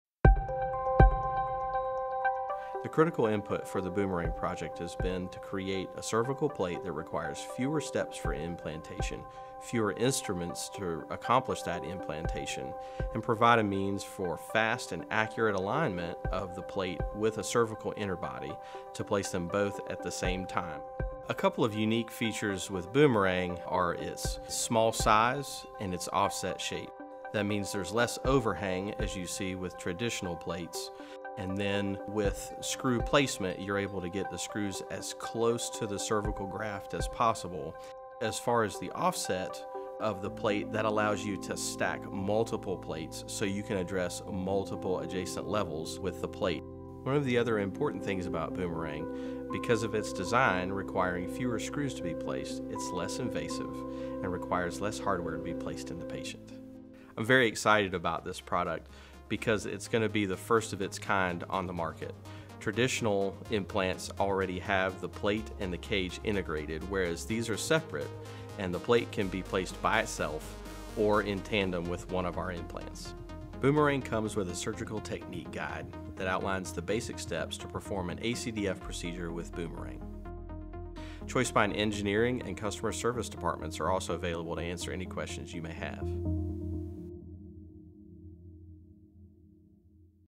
ChoiceSpine's Boomerang™ Mimicks a Standalone ACDF Concept with Synchronized Insertion
Boomerang™ is simplifying ACDF procedures through optimal design and streamlined instrumentation.

• Synchronized insertion
• Two-screw configuration
• Low-profile with offset design